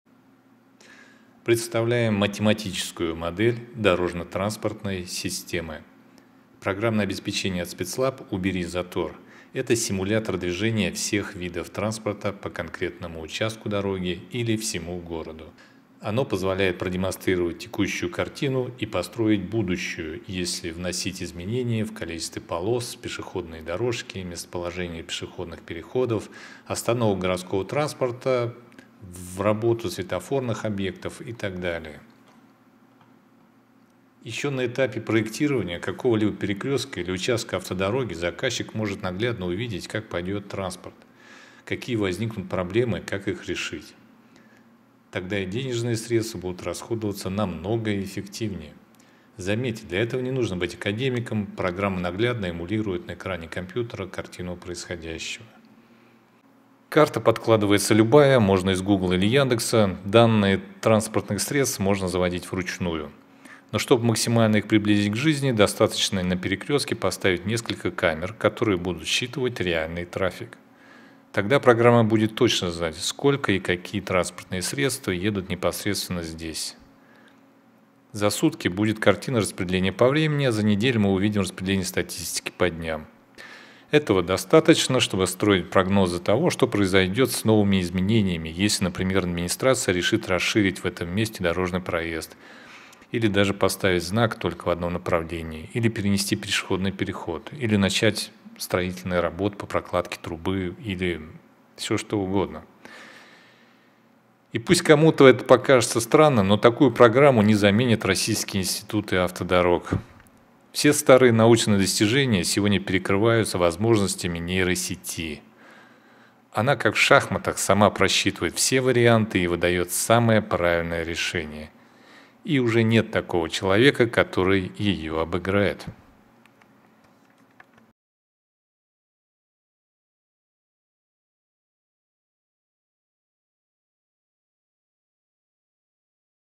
Математическая модель автотранспортных магистралей
Математическая модель дорожно-транспортной системы

ПО от Спецлаб «Убери затор» - это симулятор движения всех видов транспорта по конкретному участку дороги или всему городу.

Оно позволяет продемонстрировать текущую картину и построить будущую, если внести изменения в количество полос, пешеходные дорожки, местоположение пешеходных переходов, остановок городского транспорта, в работу светофорных объектов…